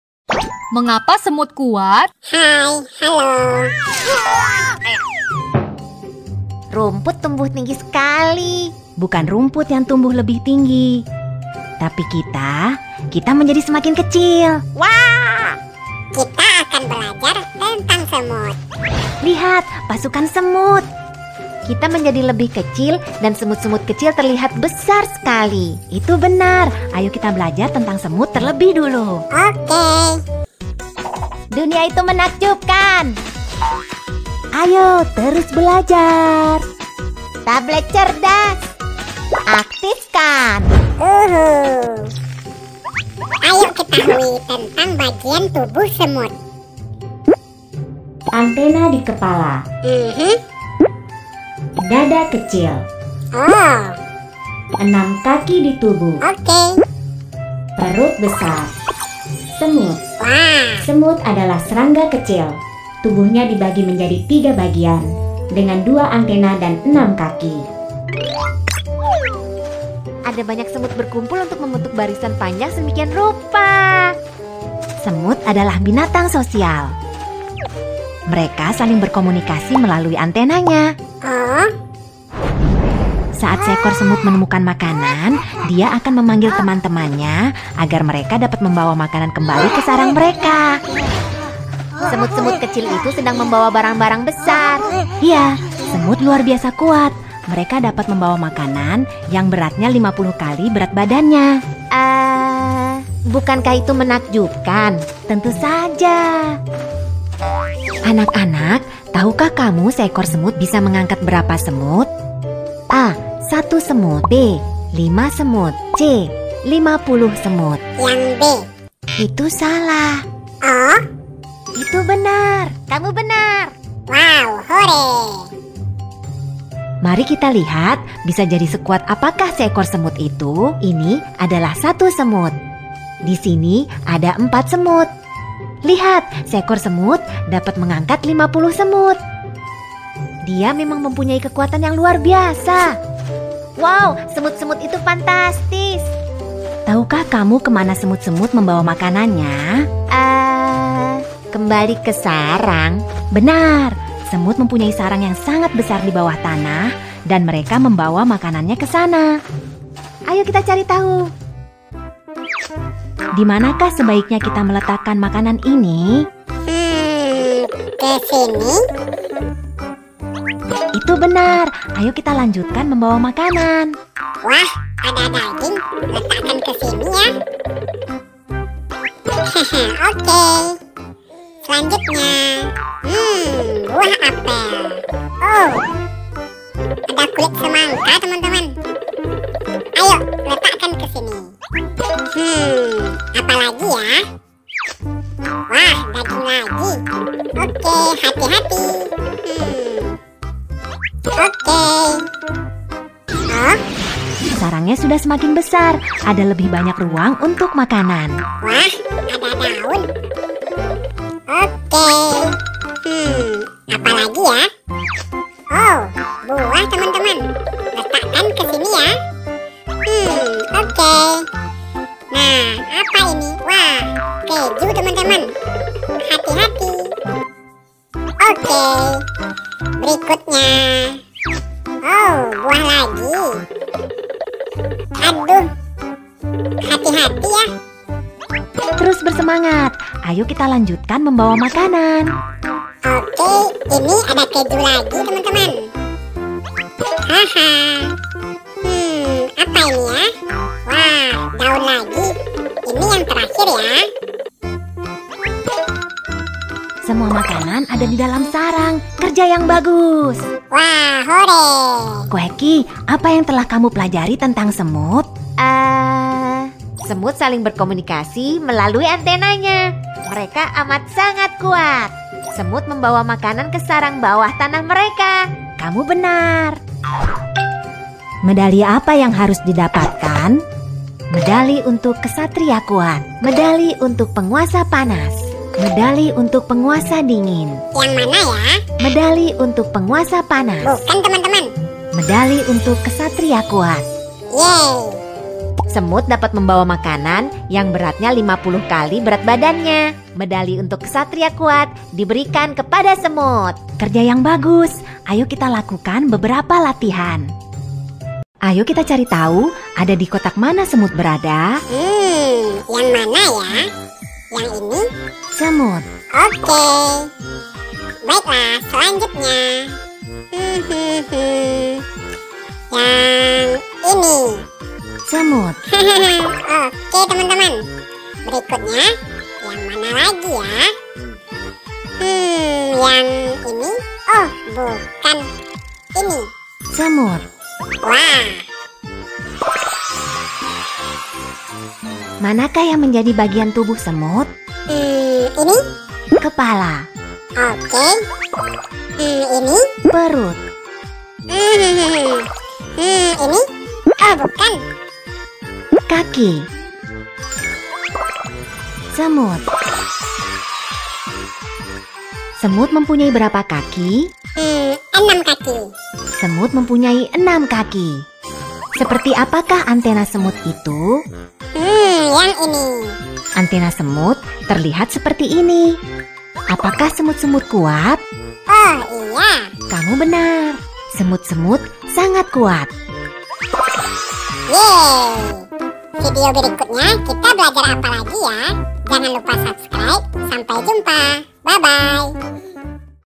Ternyata Semut Adalah Hewan yang Kuat | Video Anak | Game Anak
Selamat Datang di Channel Hawhau - Video Anak Anak.

Pada Video Kali ini Mari Kita Belajar tentang Semut.
Ternyata Semut adalah Hewan yang Kuat Loh..!!

Apa saja Bagian - Bagian Tubuh Semut, dan Bagaimana mereka bisa dikatakan Kuat.

Ayo Tonton Videonya Sampai Habis ya.!